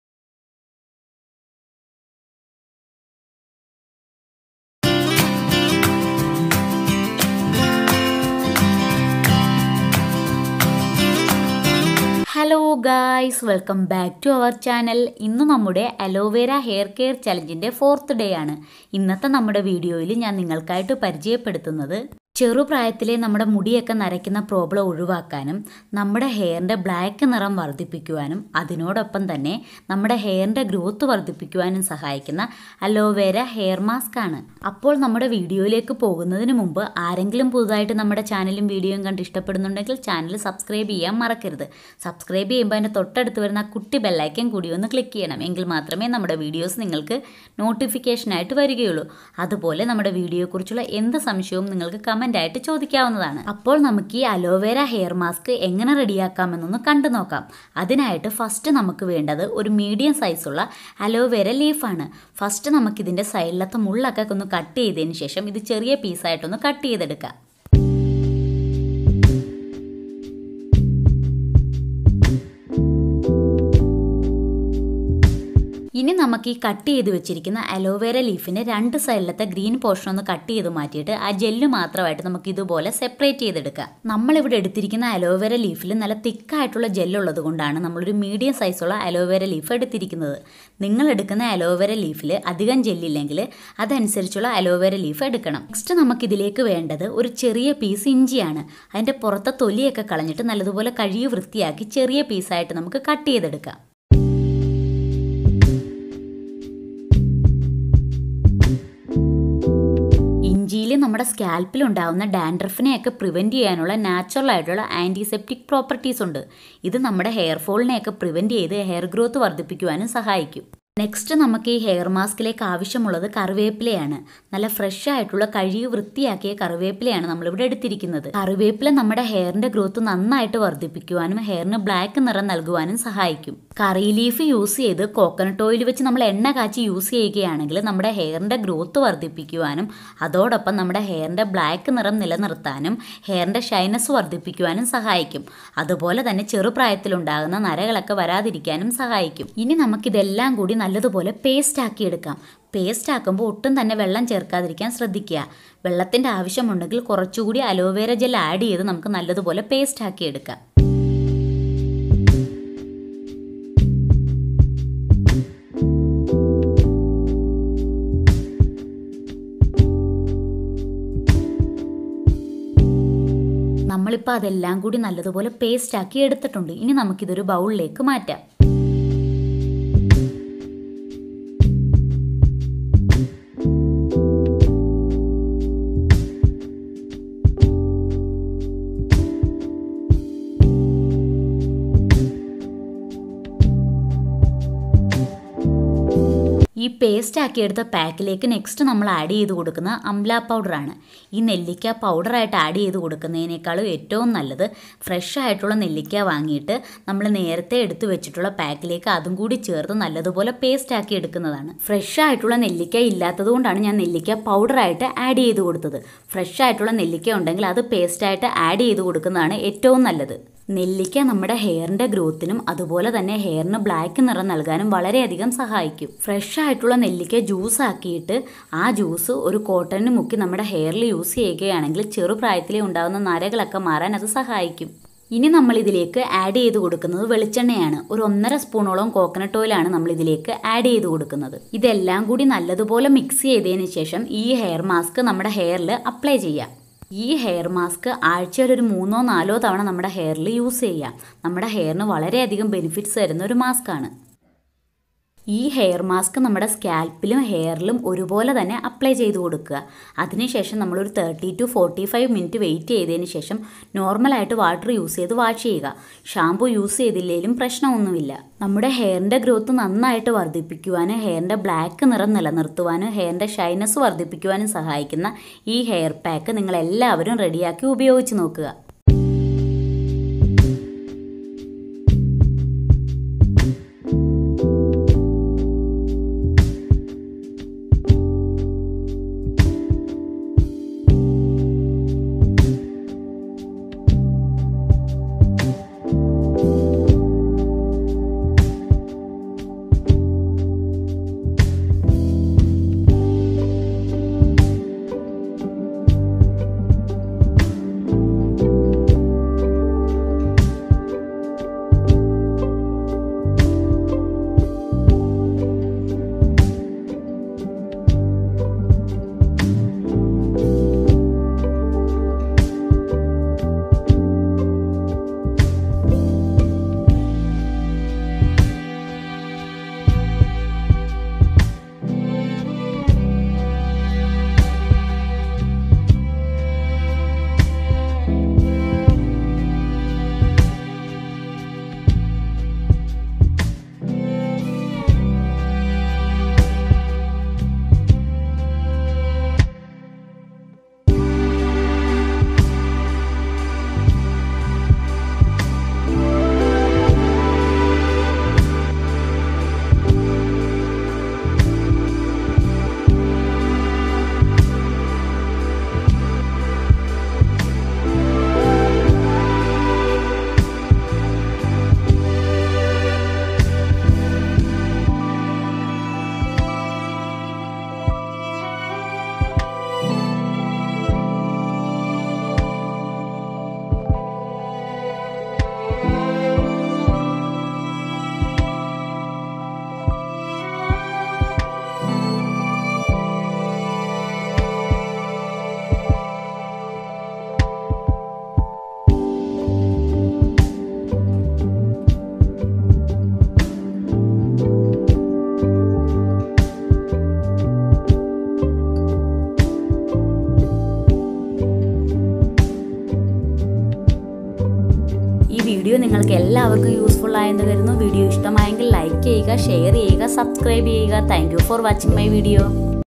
Aloe Vera NaturalHair Mask For Black Long Hair|Prevent Premature Hair Greying&Fast Hair Growth|Vlg92
Aloe Vera Natural Hair Mask For Black Long Hair|Prevent Premature Hair Greying And Fast Hair Growth

One Week Aloe Vera Hair Care Challenge

I am using Tripod

Hai Friends 🙋

"Beauty butterfly" I am Thumbi. today I am going to introduce Diy Aloe Vera hair mask for black long hair
1.Aloe vera gel
2.Curry leaves
3.Ginger
4.Amla Powder
5.Cocounut oil
this all ingredients use to making Aloe Vera hair mad for long black hair
prevent Premature greying hair

ONE WEEK TOMATO  SKIN CARE CHALLENGE|

☑️☑️Always take patch test before applying anything on your face.☑️☑️

***Please do not use any photos or content with out permission.